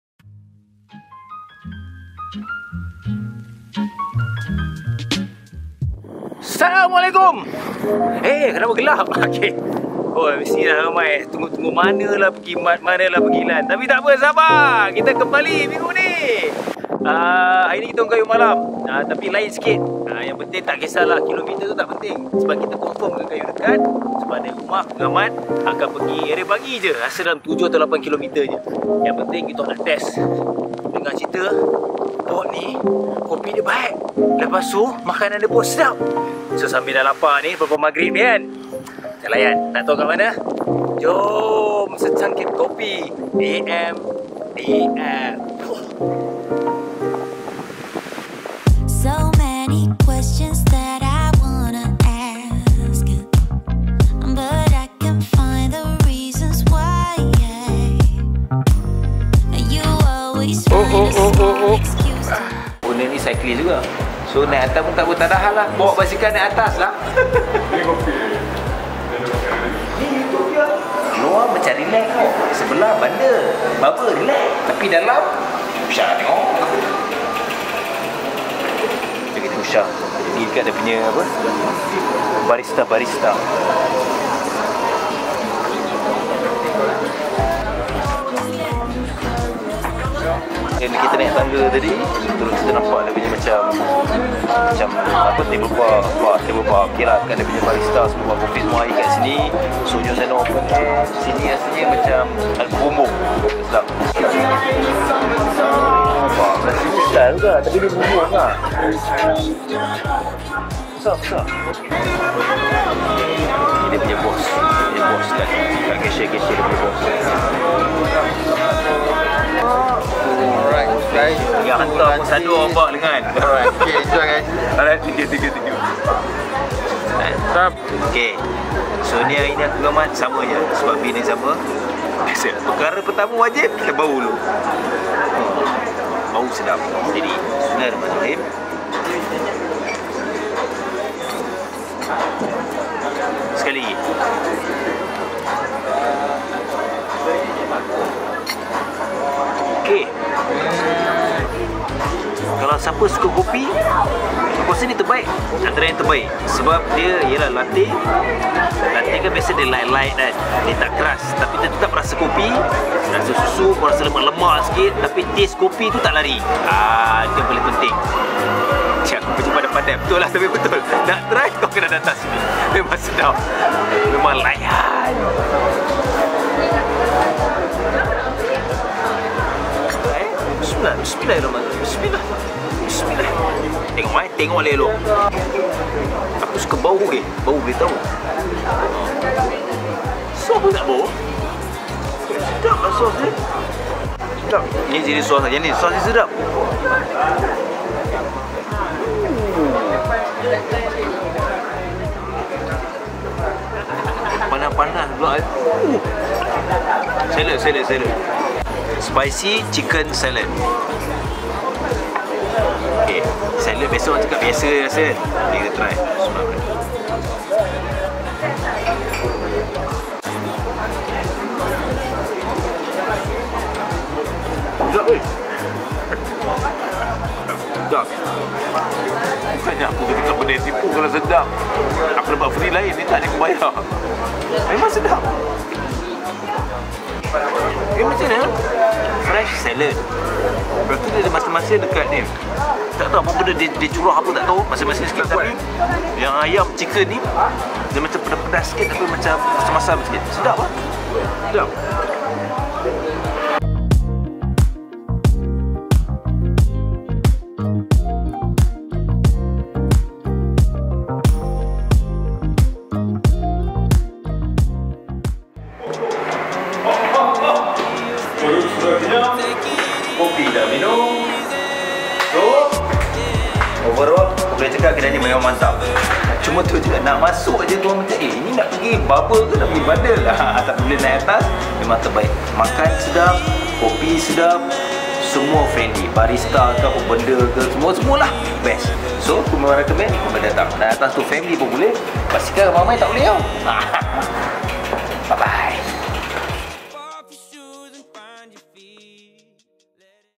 Secangkir Kopi Ride Bersama AM & AF (E4)
Ride.Coffee.Ride. We cycle from home to any coffee shop in Kuala Lumpur. In this episode, we went to Denali & Pinnacles, Bandar Baru Bangi Seksyen 3.